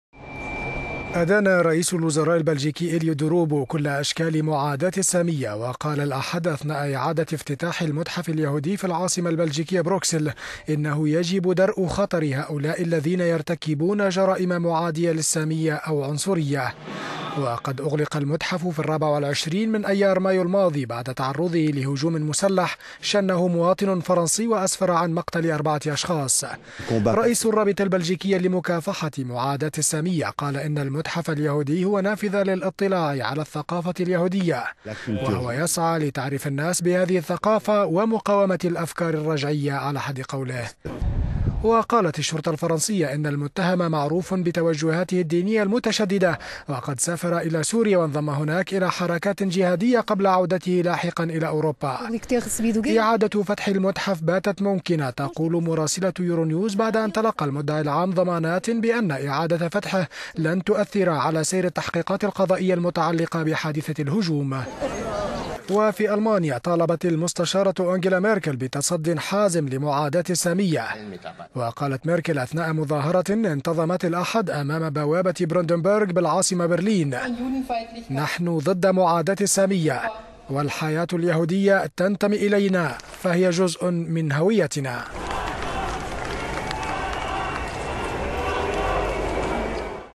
إعادة افتتاح المتحف اليهودي في بروكسل وميركل تطالب بالتصدي لمعاداة السامية
أدان رئيس الوزراء البلجيكي إيليو دي روبو كل أشكال معاداة السامية و قال الأحد أثناء إعادة افتتاح المتحف اليهودي في العاصمة البلجيكية بروكسل إنه يجب درء خطر هؤلاء الذين يرتكبون جرائم معادية
للسامية أو عنصرية.
وقد أغلق المتحف في الرابع والعشرين من آيار/مايو الماضي بعد تعرضه
لهجوم مسلح شنه مواطن فرنسي وأسفر عن مقتل أربعة أشخاص.

رئيس الرابطة البلجيكية لمكافحة معاداة السامية قال إن المتحف اليهودي هو نافذة للاطلاع على الثقافة اليهودية وهو يسعى لتعريف الناس بهذه الثقافة ومقاومة الأفكار الرجعية."